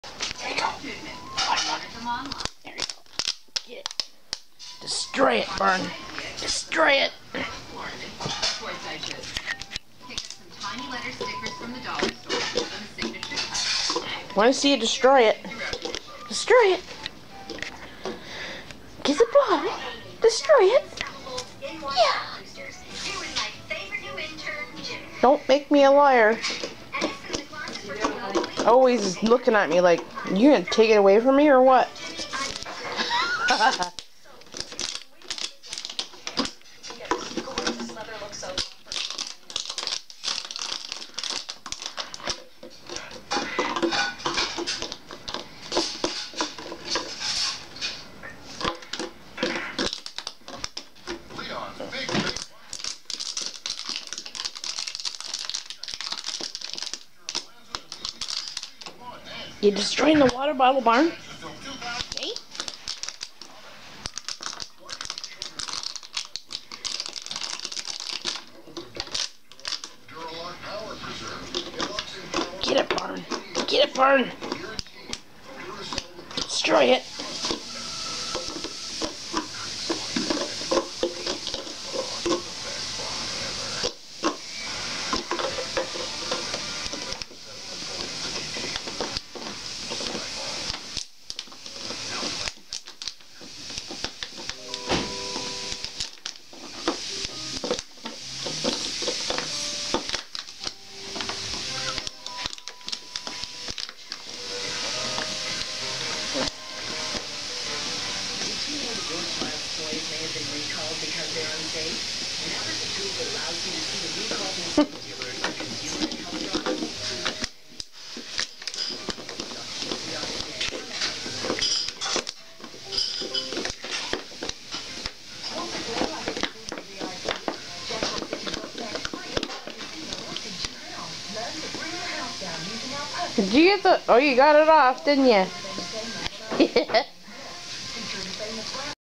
Pembroke Welsh Corgi Barney crushing an empty water bottle-2012
My moms corgi Barney loves taking the empty water bottles and crushing them up when he puts a hole in em we take em away though but he loves doing this!